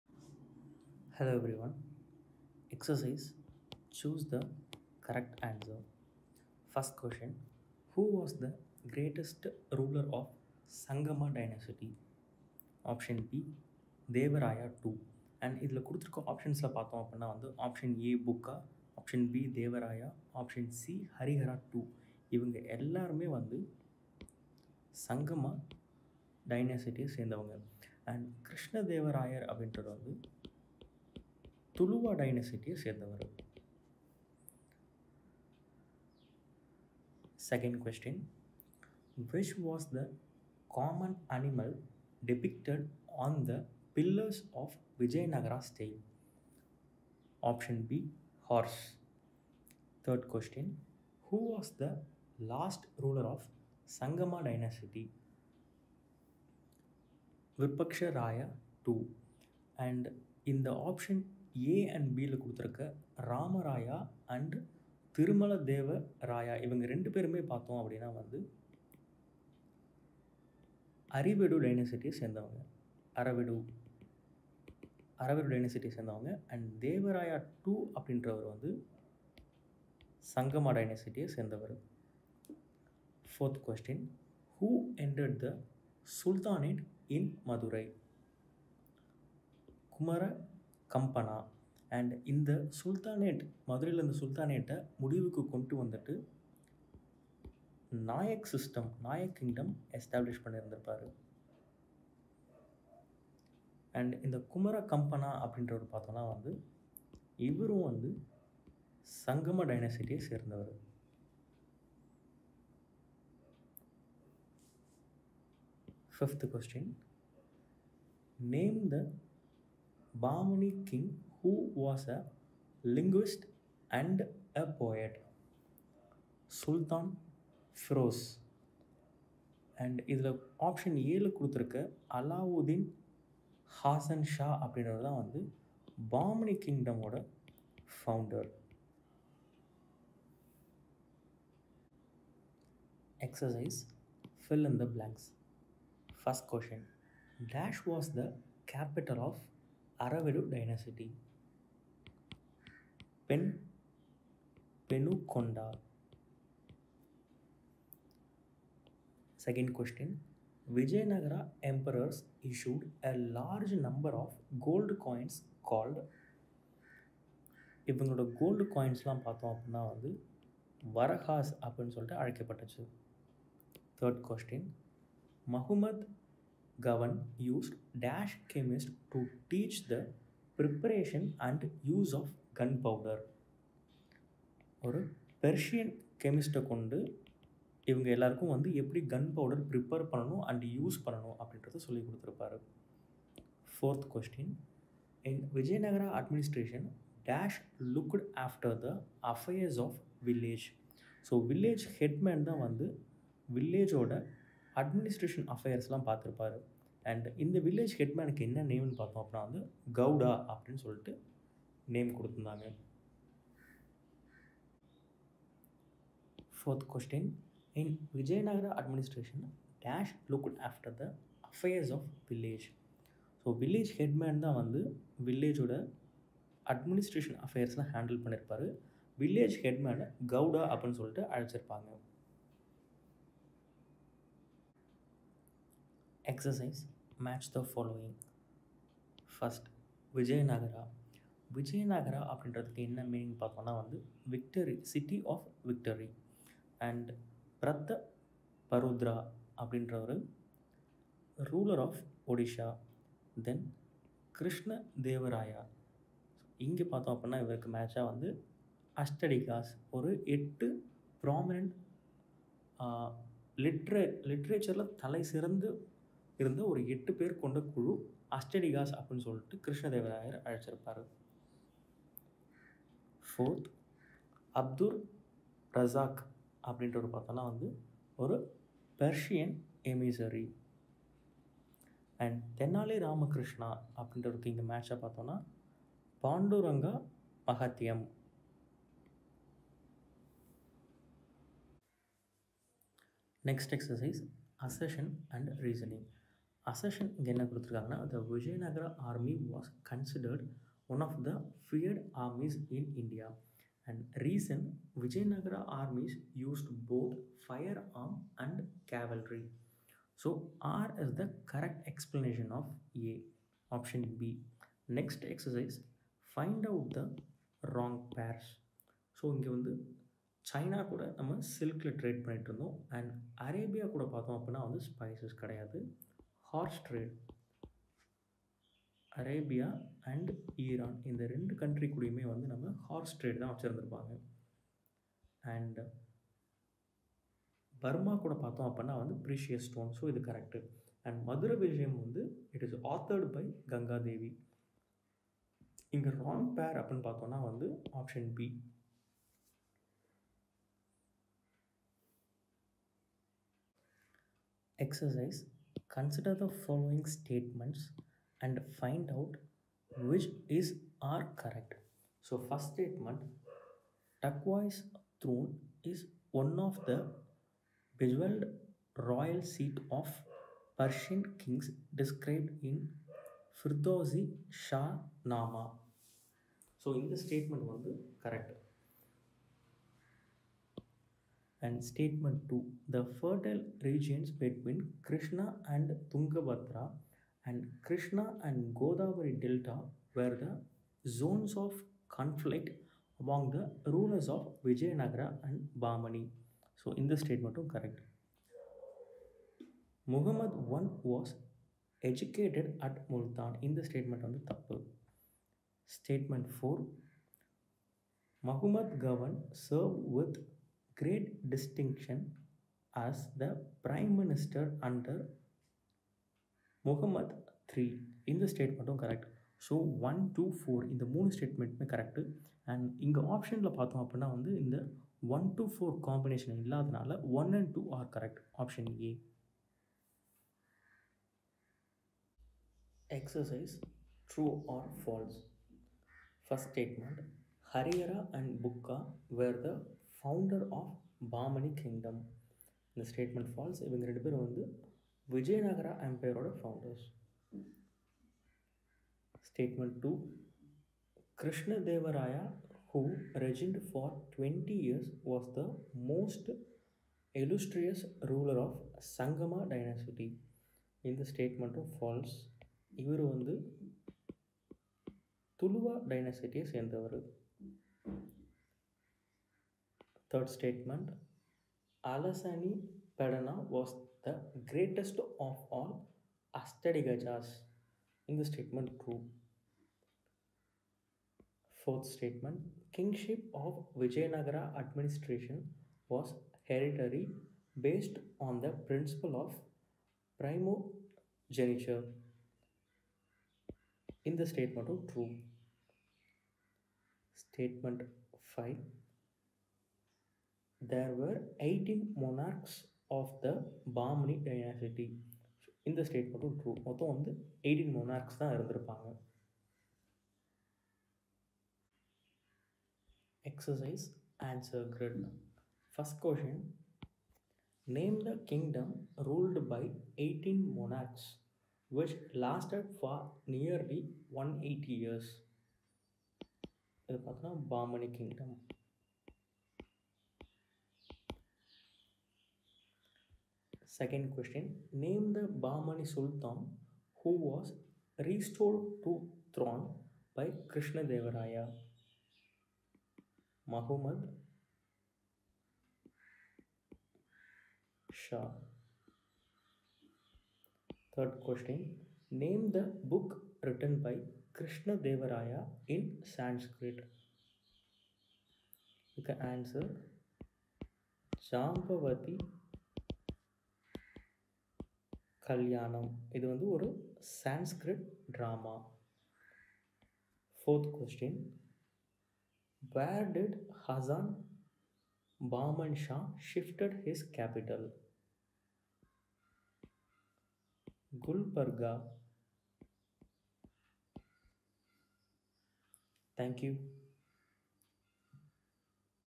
7th std - Term 2 | Social Unit 1 | Vijayanagar and Bahmani Kingdom | Book Back Answers | withak
0:00 Choose the correct answer
2:23 Fill in the blanks
3:55 Match the following
4:54 IV Exercise
7:13 True or False
8:33 Answer Grid